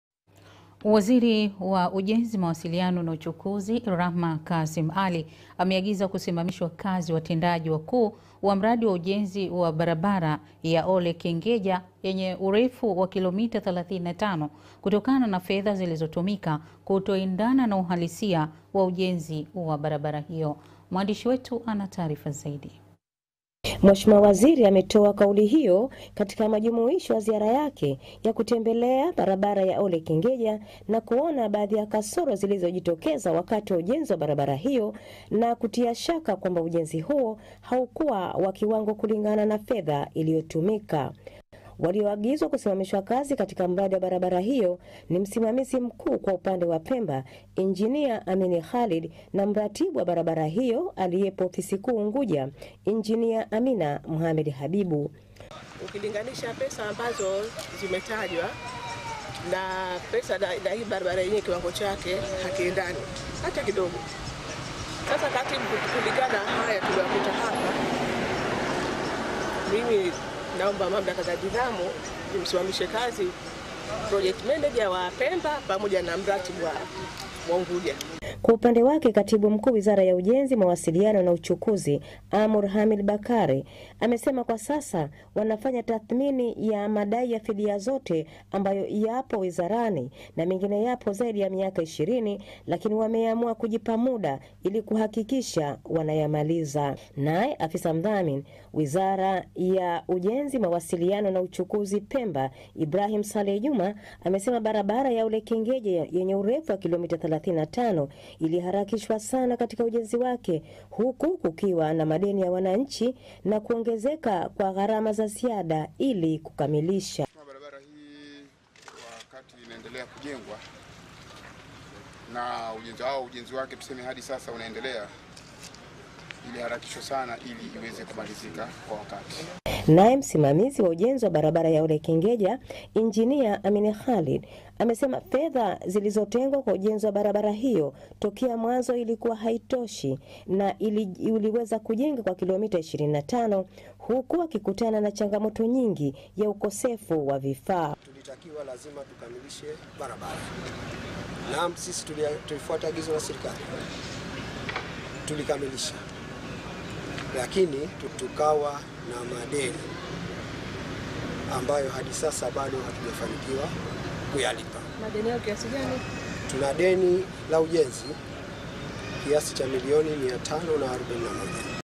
kusimamishwa
15/3/2021
WAZIRI waUjenziMawasilianonaUchukuziRahmaKassim Ali, ameagizakusimashwakaziwatendajiwakuuwamradiwaujenziwabarabaraya Ole-KengejayenyeurefuwaKilomita 35, kutokananafedhazilizotumikakutokuendananauhalisiawaujenziwabarabarahiyo.
 MH: RAHMA iametoakaulihiyokatikamajumuishoyaziarayakeyakutembeleayabarabaraya Ole-Kengeja, nakuonabaadhiyakasorozilizojitokezawakatiwaujenziwabarabarahiyonakujengashaka, kwambaujenzihuohaukuwawakiwangokulingananafedhailiyotumika.
Walioagizwakusimamishwakazikatikamradiwabarabarahiyo, niMsimamizimkuukwaupandewa Pemba InjiniaAmini Khalid naMratibwabarabarahiyoaliyepoafisikuuUngujaInjiania Amina Mohamed Habibu.
. Kwa upande wake katibuMkuuWizarayaUjenziMawasilianonaUchukuzi AmourHamil Bakar, alisemakwasasawanafanyatathminiyamadaiyafidiazoteambayoyapowizaraninamengineyapozaidiyamiaka 20, lakiniwameamuakujipamudailikuhakikishawanayamaliza.
NayeafisamdhaminiWizarahiyoIbarhami Saleh Juma, amesemabarabaraya ole – kengejayenyeurefuwa  km 35  ilkiharakishwasanakatikaujenzi wake   hukukukiwanamadeniyawananchinakuongezekakwagharamailikukamilisha
NayeMsimamiziwaujenziwabarabaraya Ole-Kengeja, InjiniaAmini Khalid, alisemafedhailiyotengwayaujenziwabarabaratokamwanzoilikuahaitoshinailiijengakwakilomita 25, hukuakikutananachangamotonyingiyaukosefuwavifaa.
Kwa upandewaowananchikatikaeneo la Pujini, wamesemamvuazinaponyeshamajiyotehuingiakatikanyumbazaonakusababishakerokwao.
Mwisho